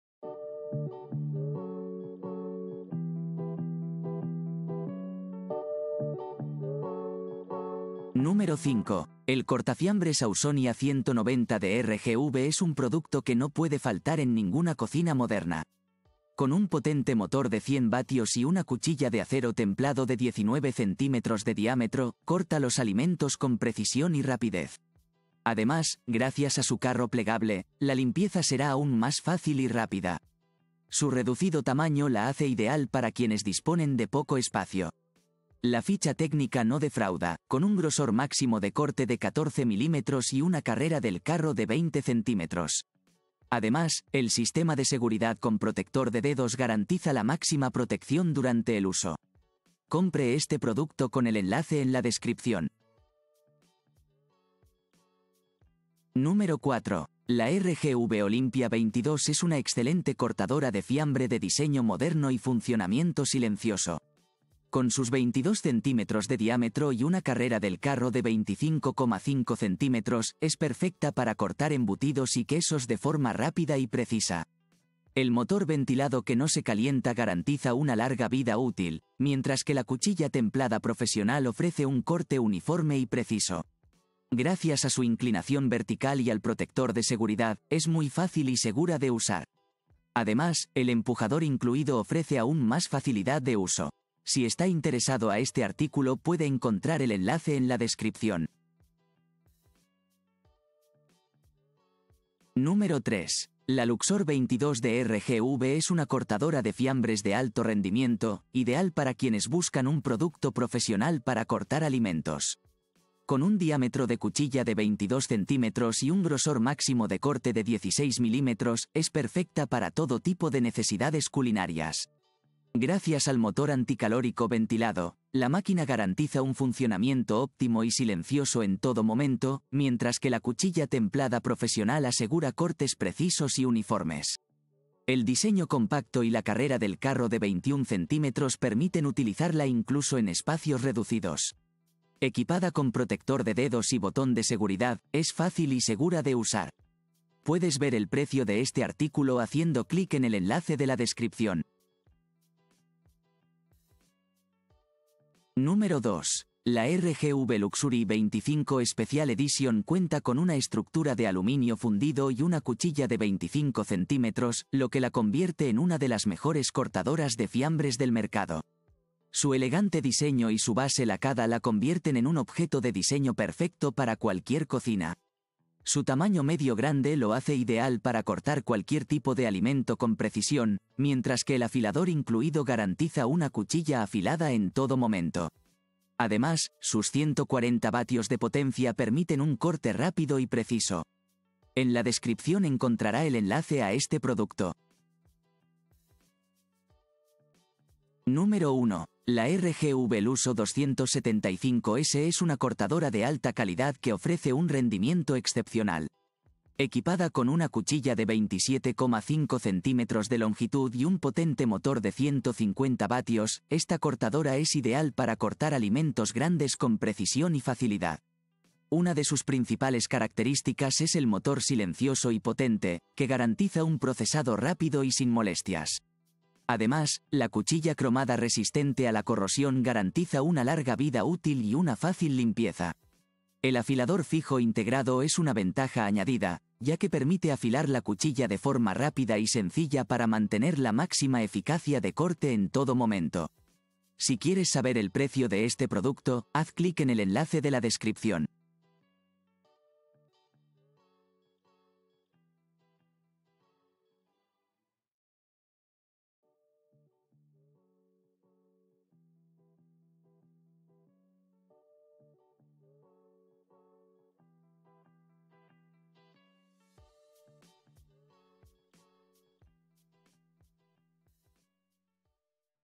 Las 5 mejores cortadoras RVG
1) RGV LUSSO 275/A Eléctrico... » Amazon
2) RGV 25 - Cortadora de fia... » Amazon
3) RGV Luxor 22 Eléctrico 12... » Amazon
4) RGV - R. G.V. 22GS Lujo C... » Amazon
5) RGV Ausinia 190 Eléctrico... » Amazon

✋ Aquí están las 5 mejores cortadoras rvg, disponible en el mercado.

00:00 - Intro
00:08 - 5) RGV Ausinia 190 Eléctrico 100W Aluminio Plata rebanadora - Cortafiambres (Alumin...
01:02 - 4) RGV - R. G.V. 22GS Lujo Cortadora, Diámetro de 22 Cm.
01:45 - 3) RGV Luxor 22 Eléctrico 120W Aluminio Plata rebanadora - Cortafiambres (Aluminio,...
02:38 - 2) RGV 25 - Cortadora de fiambres edición especial, color rojo.
03:24 - 1) RGV LUSSO 275/A Eléctrico 150W Aluminio Plata rebanadora - Cortafiambres (Alumin...